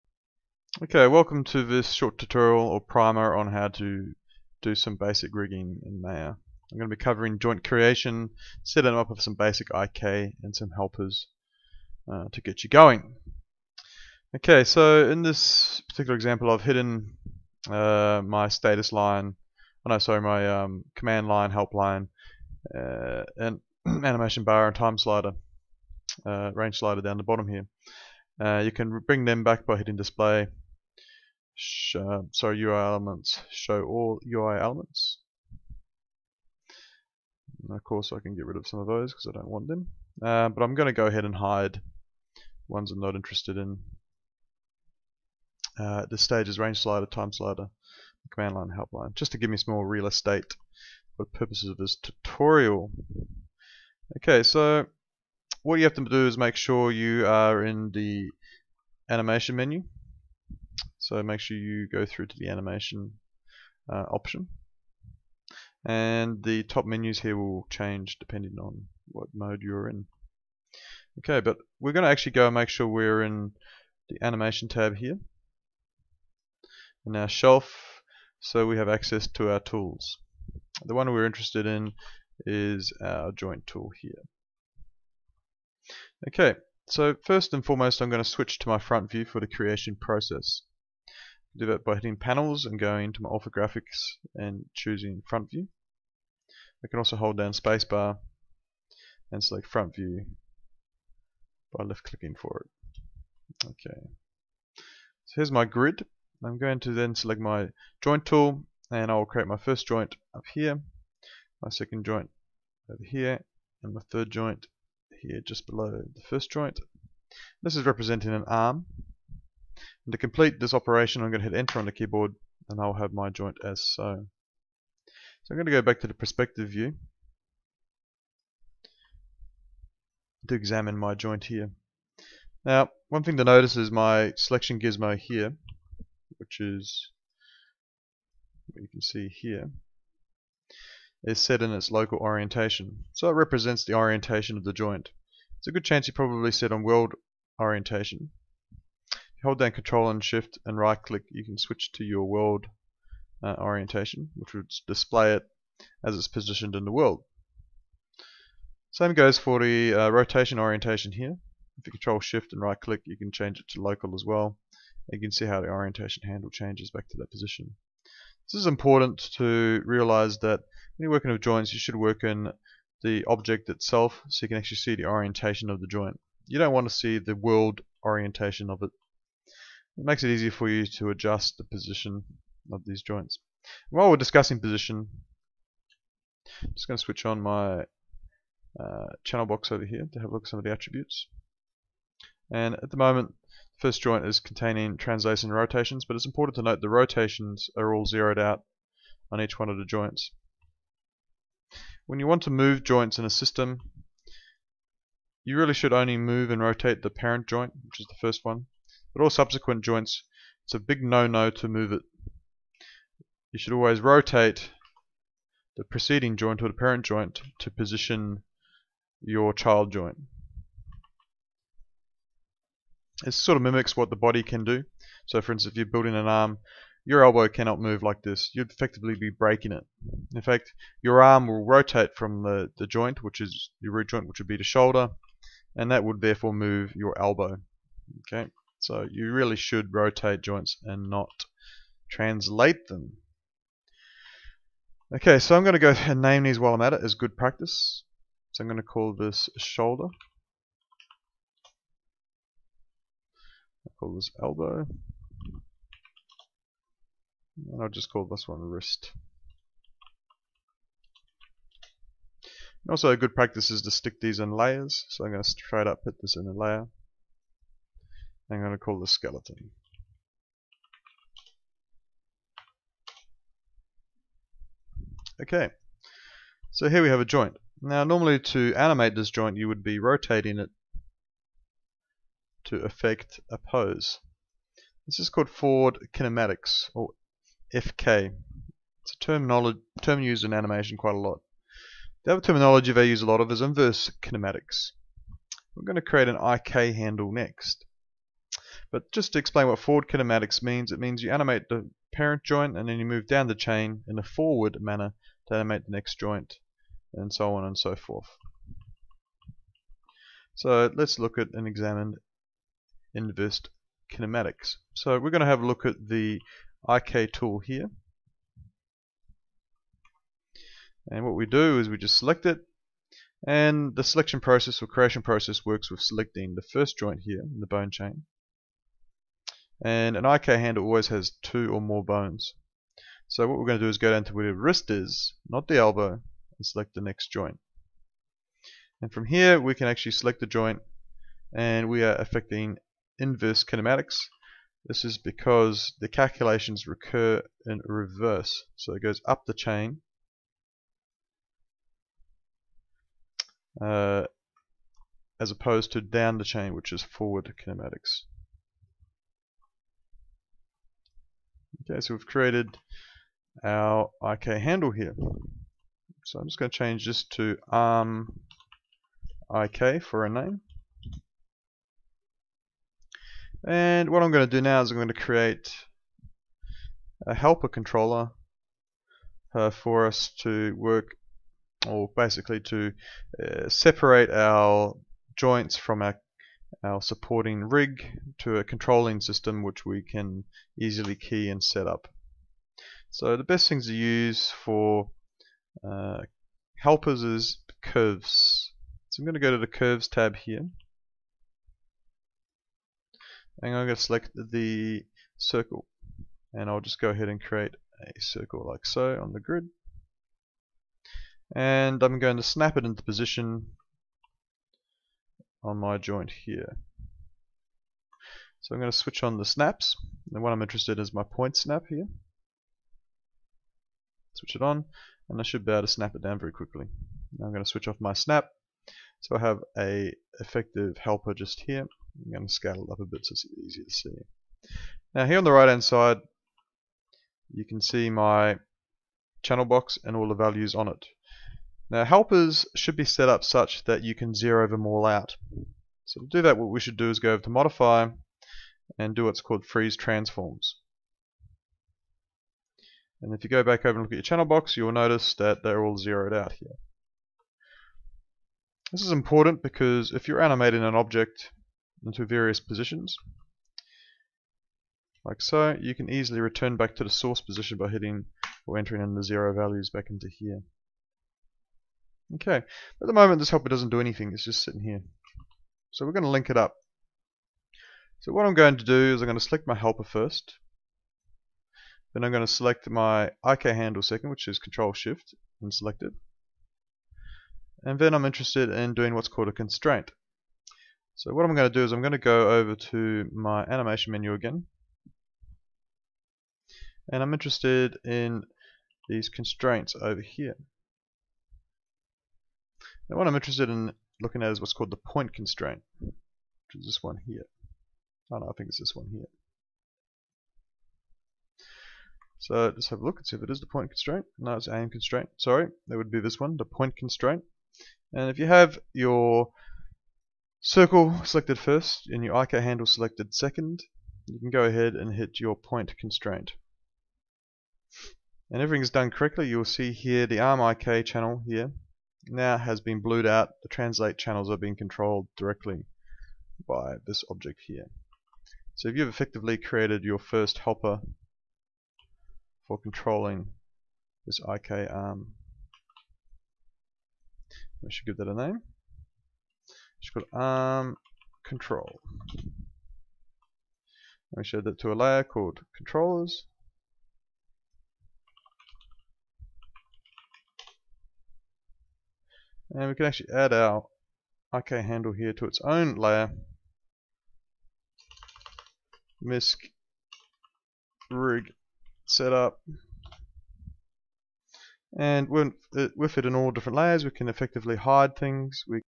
Primer in rigging in Maya - Part 1 of 2
In this two part series I'll step through some very basic concepts of rigging, from setting up joints, to naming, to creating IK and layers for organisation of your scene.